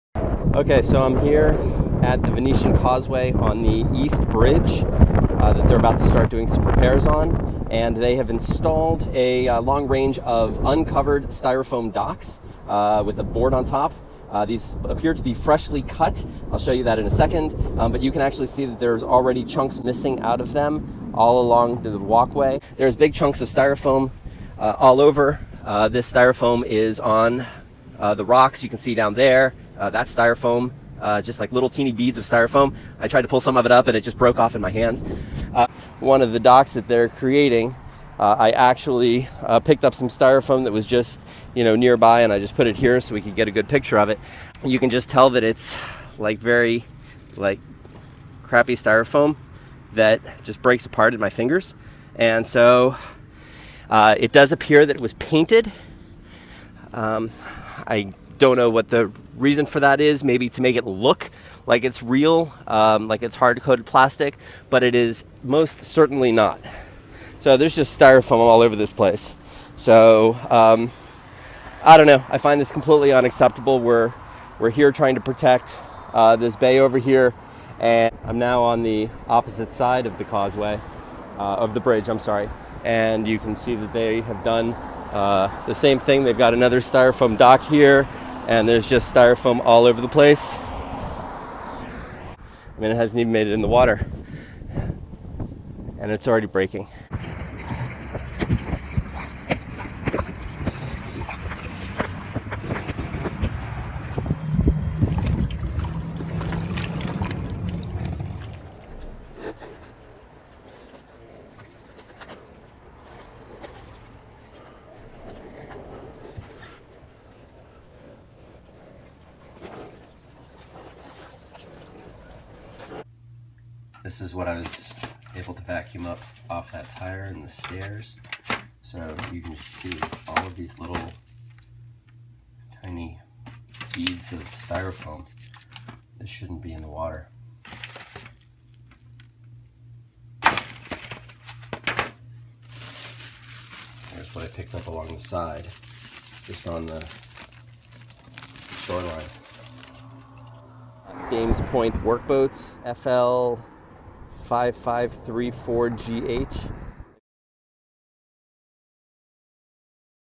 Styrofoam Docks - Venetian Causeway Miami Beach
Unecapsulated Styrofoam docks being used by KIEWIT Infrastructure South Co. as a contractor for Miami Dade County for bridge repairs. These docks are making a HUGE mess of broken foam into the bay. When adivsed, Miami Dade County forced KIEWIT to remove the foam blocks within 24 hours and encapsulate them. Why they thought it was a good idea to use in the first place is beyond me. MDC has since added language to their contracts requiring encapsulation.